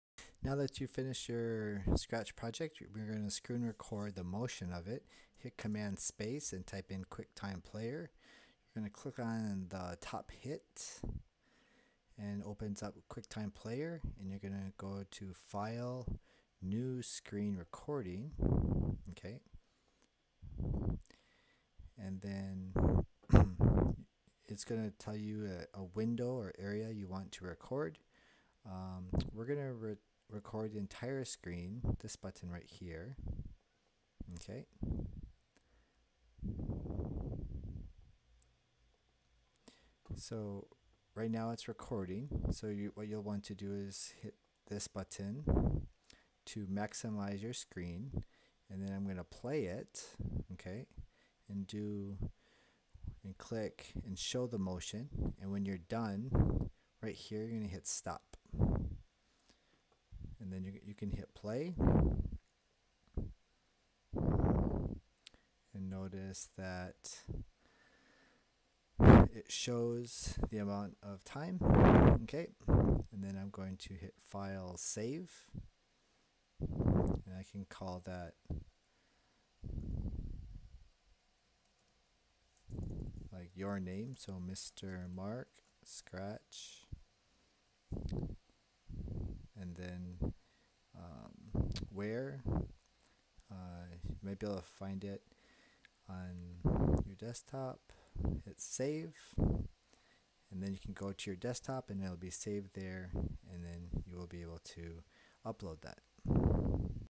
Screen Recording your Scratch Project with Apple Quicktime
In this video I'm going to show you how to save your Scratch video using Quicktime on a Mac. All scratch coding tutorials are clear and easy to follow.